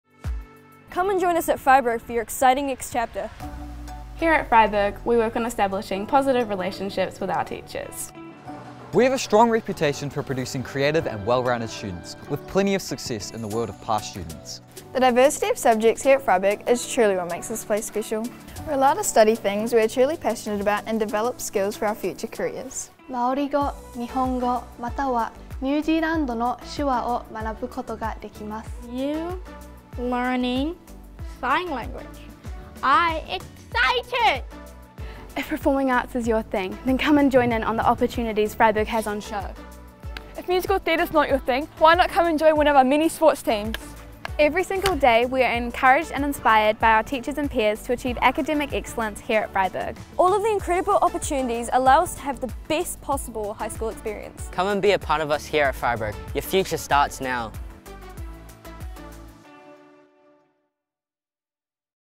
Welcome to Freyberg High School
We recently returned to our roots, helping our mates at Freyberg High School create a tasty school promo video to celebrate independence and the opportunity offered to all students.